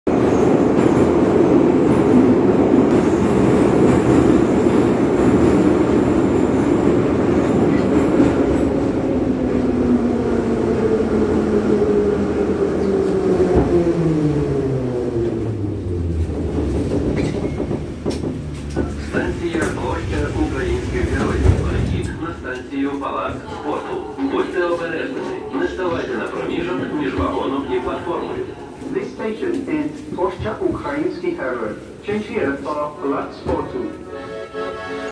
KYIV METRO new announcement | нове оголошення в метро Києва - Площа Українських Героїв
New announcement in the Kyiv metro system - station Ploscha Ukrainskykh Heroiv (Square of Ukrainian Heroes), Ukrainian: Площа Українських Героїв